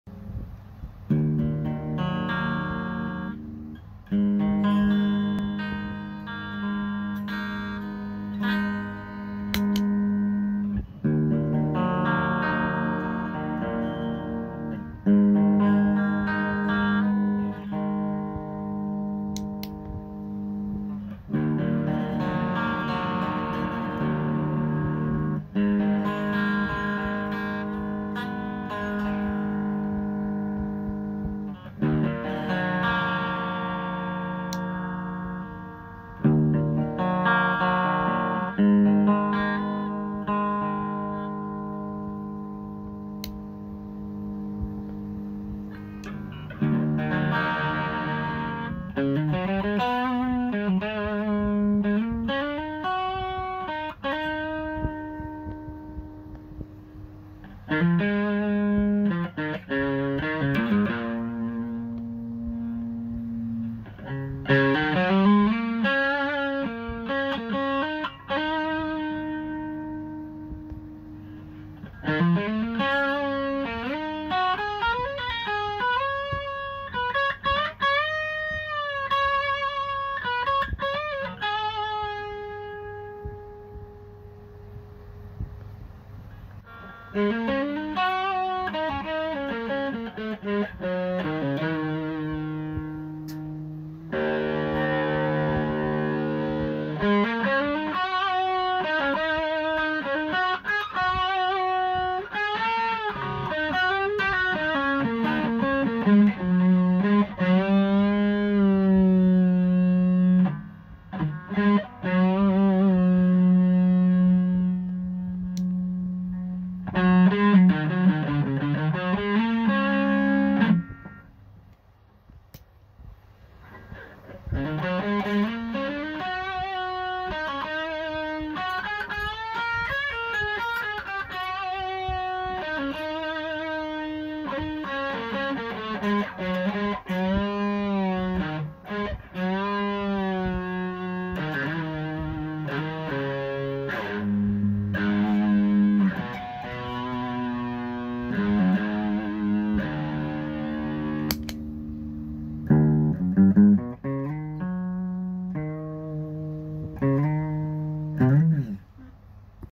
Klone silver mod
Centaur Silver edition
Bass control added
shunt diode 3 way switch
(Germanium/open/Silicone)
Dean ML
Kustom KGA16 8" combo